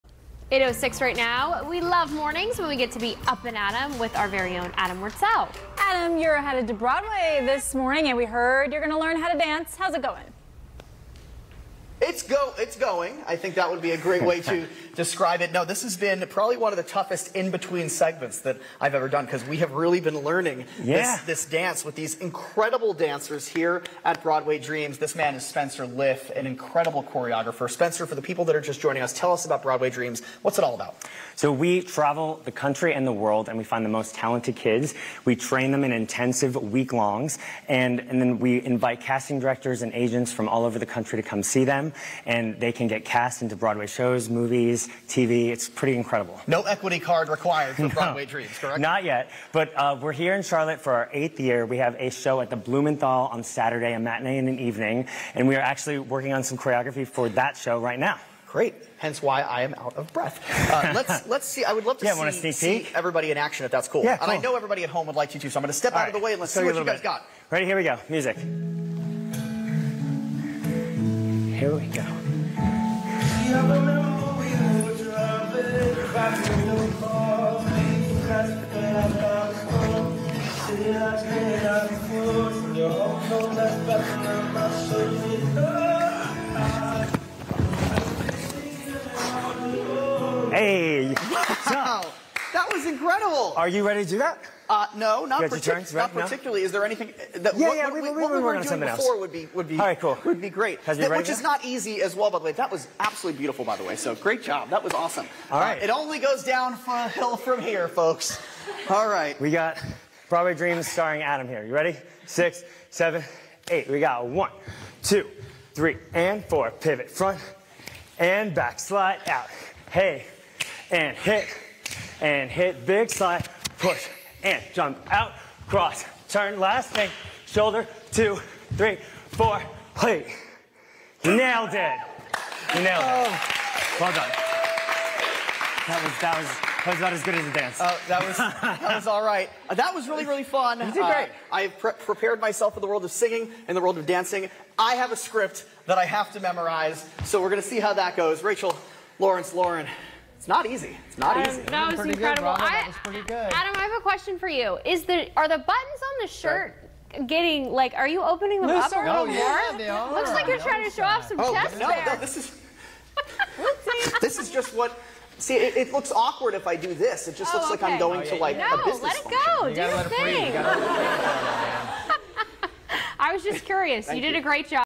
Up 'N Adam's Broadway Dreams Come True
Bahakel Entertainment's Adam Wurtzel gets a first-hand look at what it takes to be a performing actor on Broadway, thanks to non-profit organization, Broadway Dreams!